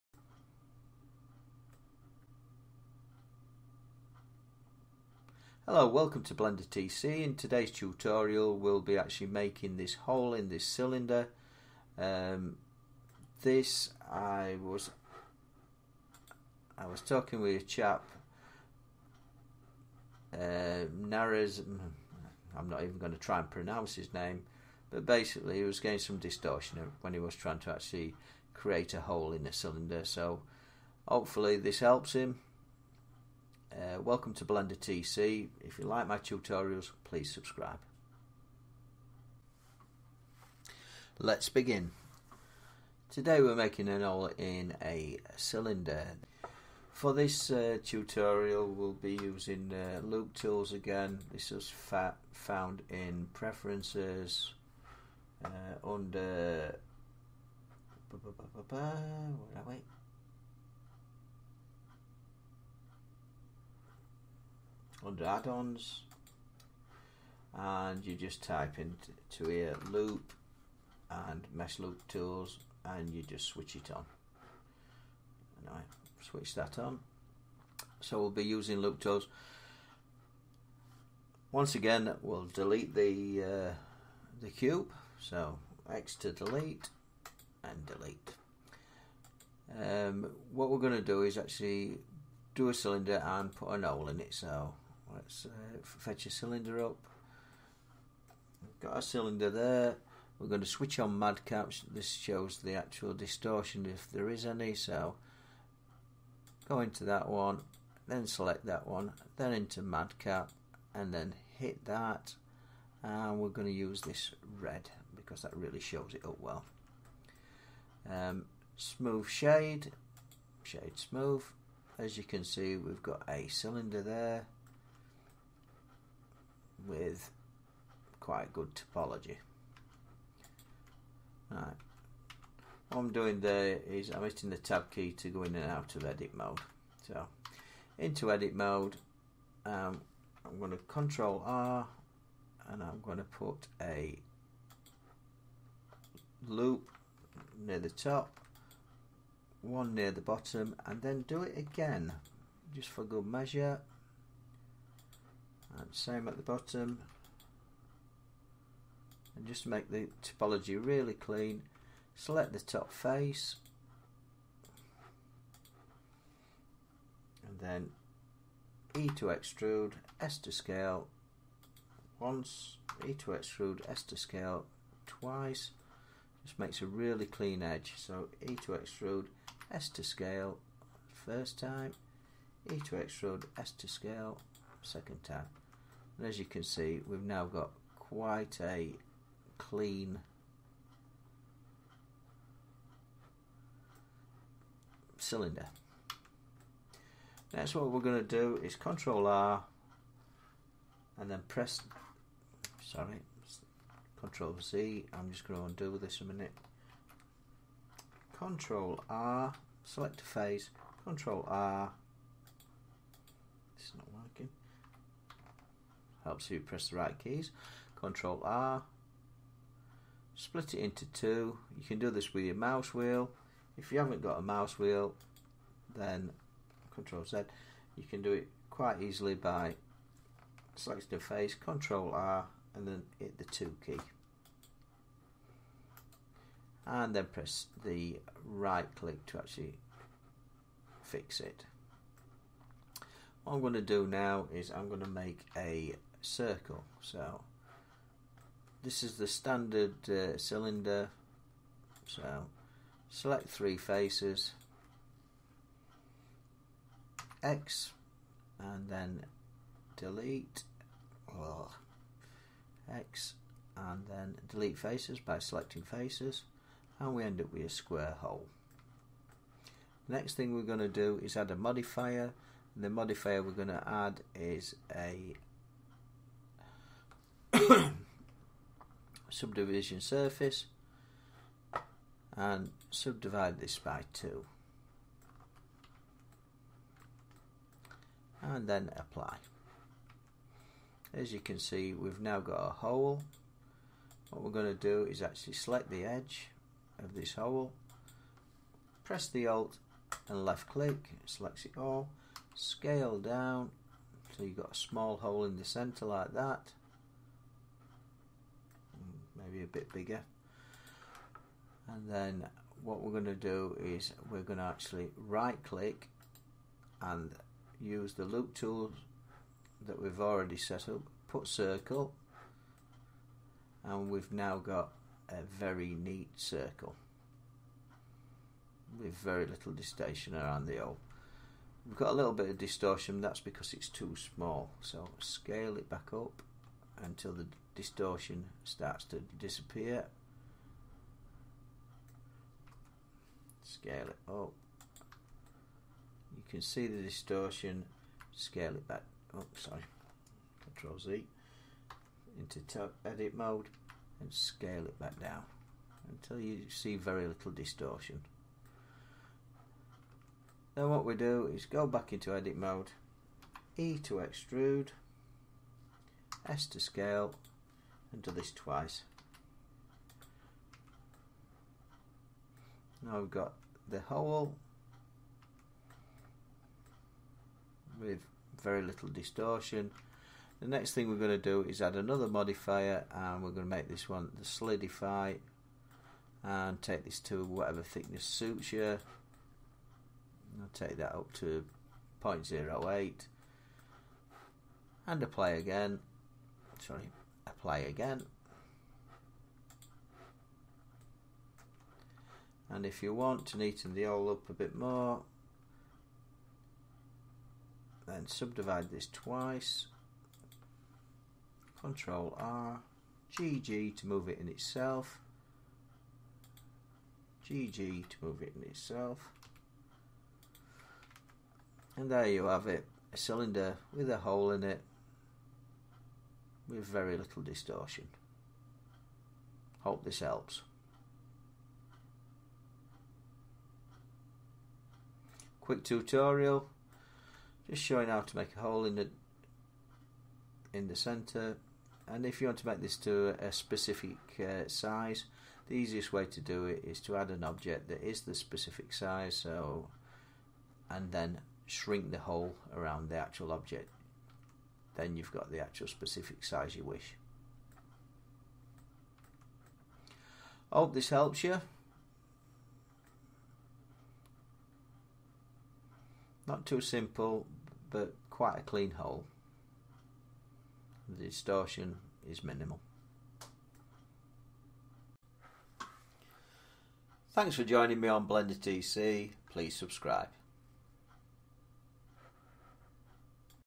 blender tutorial, how to make a hole in a cylinder, curved hole
Hello this tutorial was done to create a hole in a cylinder

Hi,
I need to make a hole (with specific diameter) in a cylinder (with specific diameter). I've read couple of posts about my problem, watched couple of tutorials but still I encounter the same problem. Even when i have - in my opinion - clean topology the hole still gives me weird shading around its sides. So, am I wrong assuming this is a clean way to do it or is a clean hole in a cylinder without shading problems impossible?